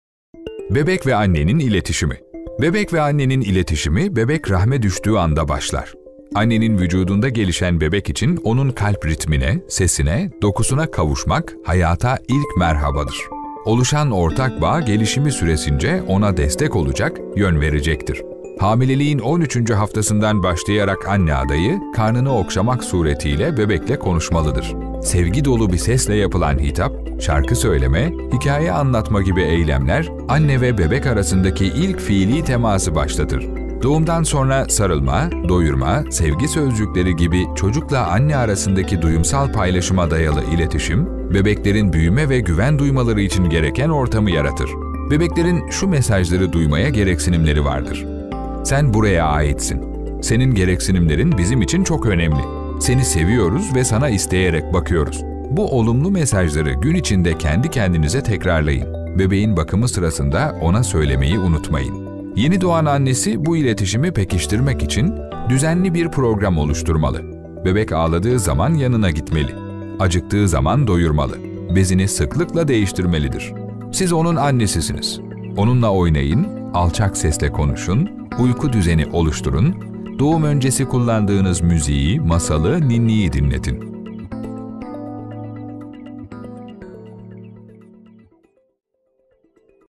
Bebek ve Annenin İletişimi
Bebek ve annenin iletişimi ne zaman başlar?
Anne ve bebek arasındaki ilk fiili temas?
Bebeklerin hangi mesajları duymaya gereksinimi vardır.
Yeni doğan anneleri neler yapmalı

Sağlık Belgeselleri